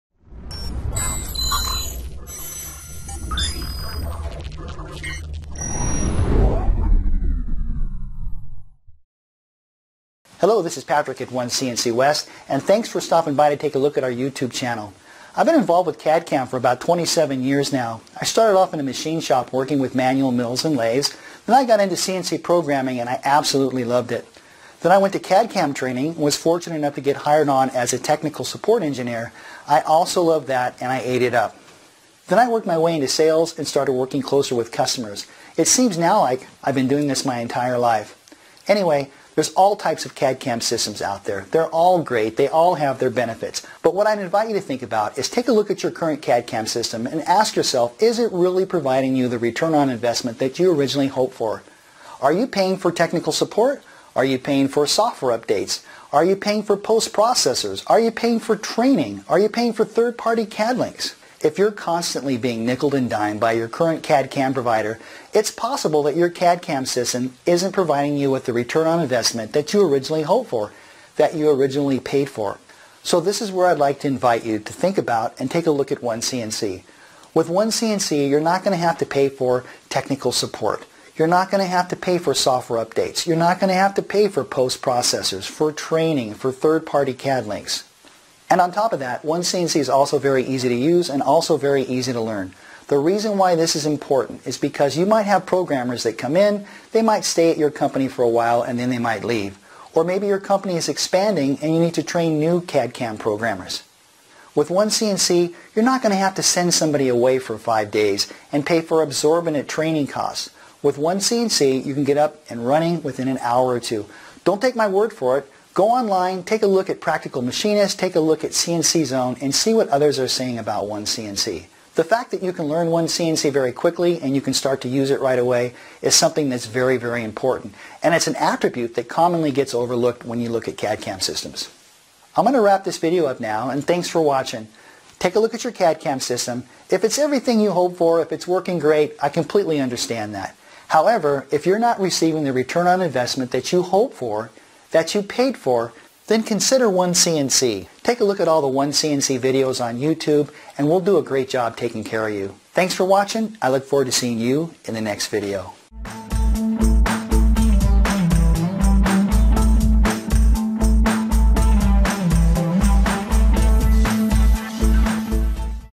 CAD CAM - OneCNC Introduction - CNC Programming
CAD CAM Video - Introduction to OneCNC CAD CAM by Patrick Matthews. CNC programming for CNC Milling Machines, Turning Machines, CNC Routers, Plasma, Waterjet and Laser.